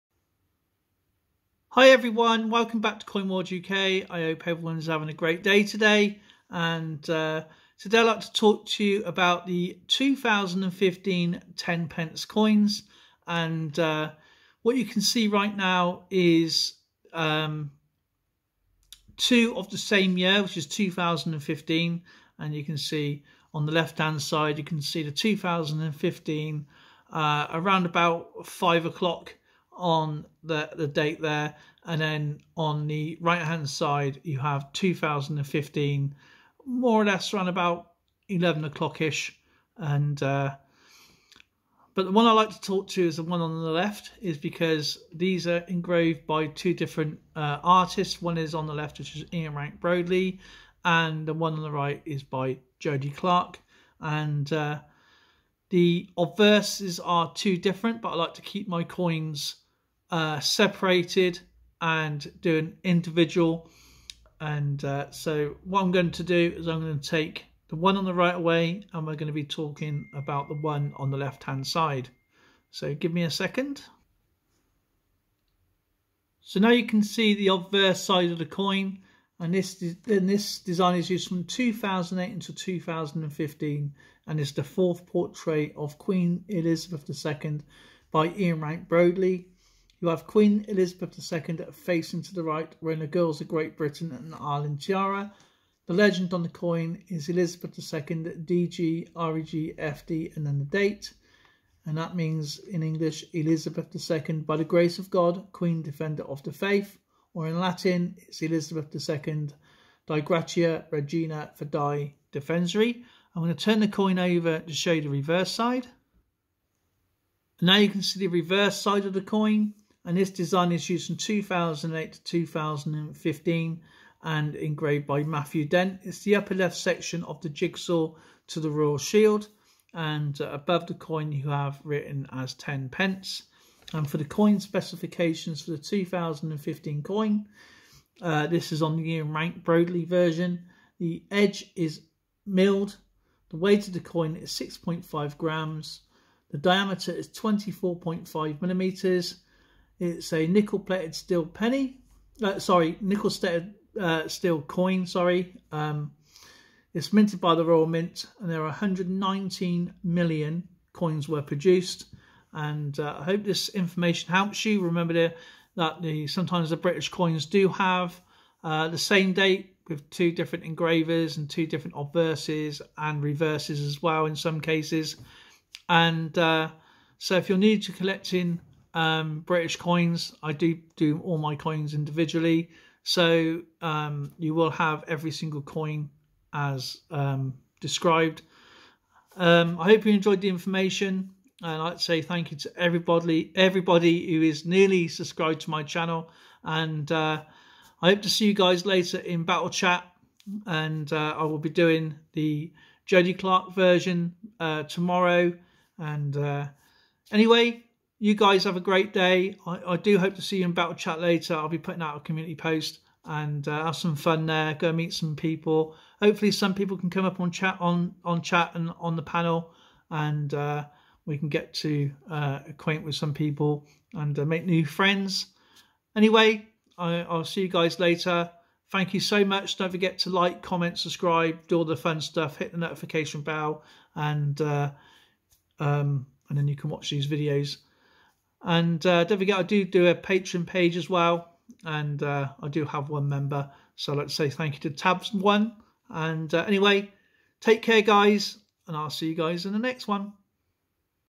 British - 2015 - 10 Pence - Small Coin - TEN PENCE - Ian Rank Broadley - Circulated - Coin World UK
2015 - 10 Pence  - TEN PENCE - Circulated
Nickel Plated Steel

Minted - Llantrisant - Wales
Mintage - 119,000,000

Ian Rank Broadley - Obverse
Matthew Dent - Reverse

Please.